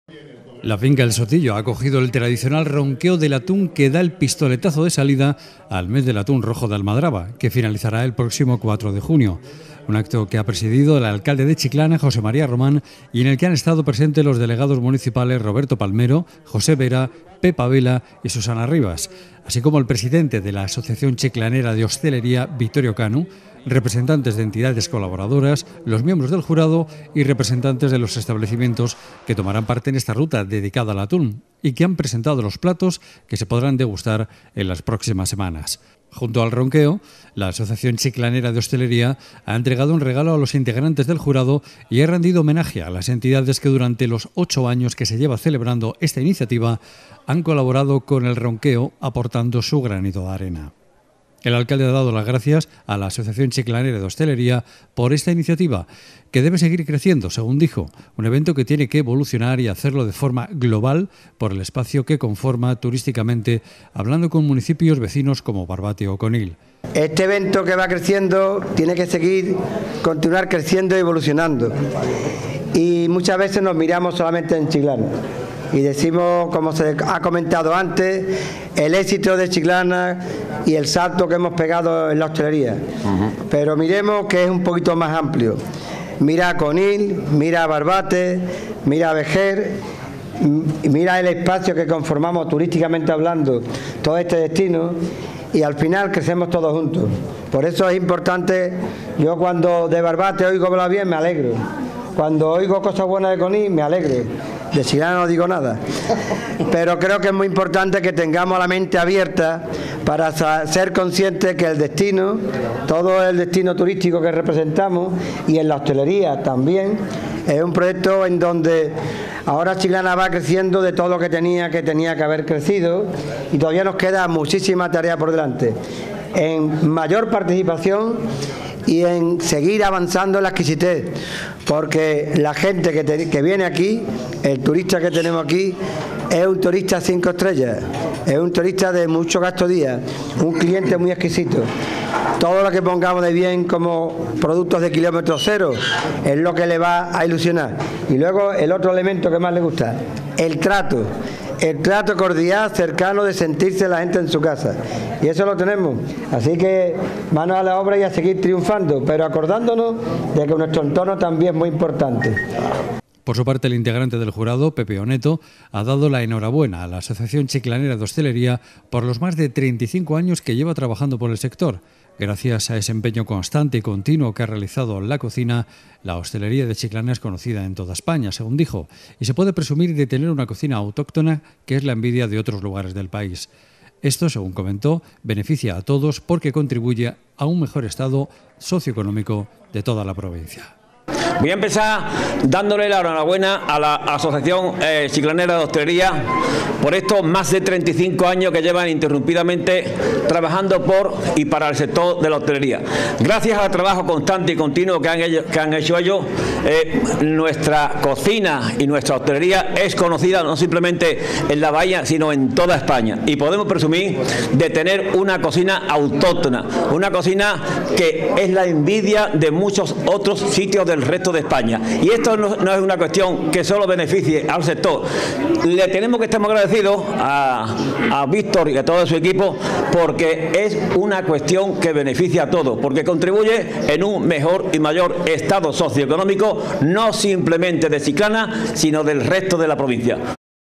EL SOTILLO ACOGE EL TRADICIONAL RONQUEO QUE DA EL PISTOLETAZO DE SALIDA AL MES DEL ATÚN EN CHICLANA
El Sotillo acoge el tradicional ronqueo que da el pistoletazo de salida al Mes del Atún Rojo de Almadraba. El alcalde preside el acto en el que se dan a conocer los diferentes platos que tomarán parte desde hoy en esta ruta dedicada al atún.